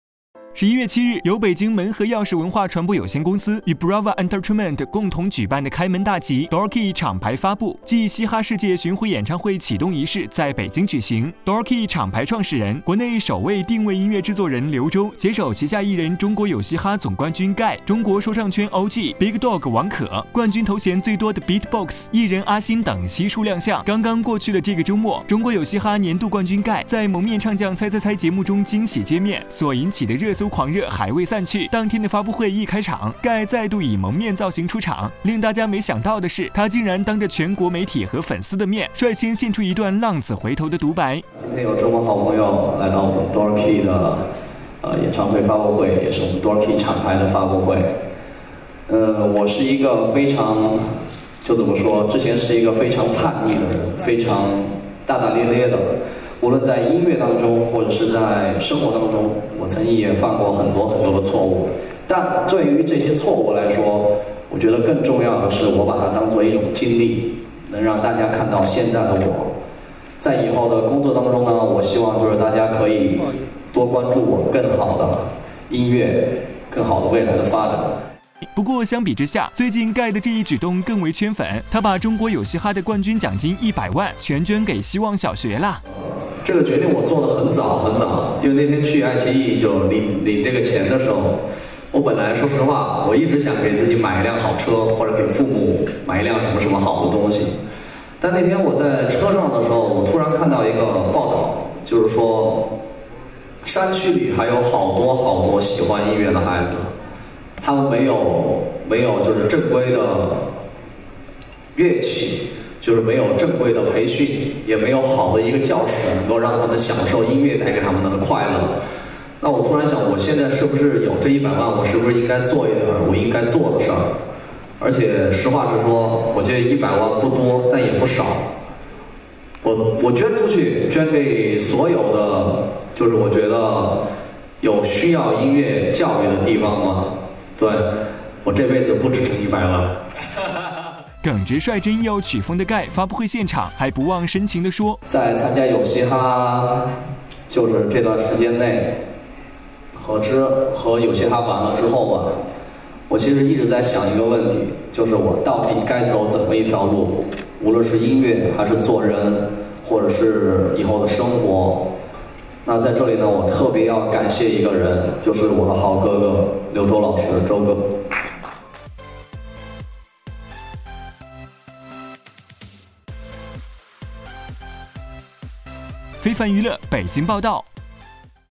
【2年前】GAI領銜全球嘻哈巡演 竟捐給希望小學100萬！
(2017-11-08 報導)

11月7日，由北京門和鑰匙文化傳媒有限公司與Bravo Entertainment共同舉辦的“開門大吉”Door&Key廠牌發佈暨嘻哈世界巡迴演唱會啟動儀式在北京舉行。